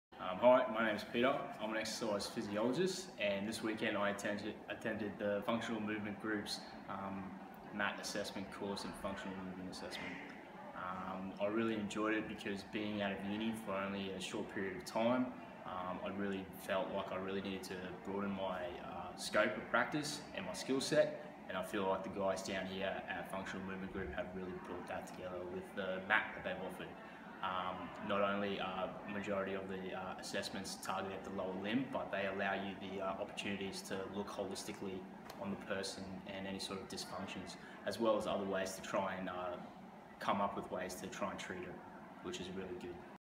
Peter Ex Phys Brisbane MAT Testimonial
"Being out of uni for a short period of time I felt I needed to extend my scope of practice and that's been really put that together with the MAT"

See what Exercise Physiologist Peter from Brisbane had to say about attending the MAT course.

To learn more about the MAT LIVE course and how it will revolutionize your assessment, treatment and training,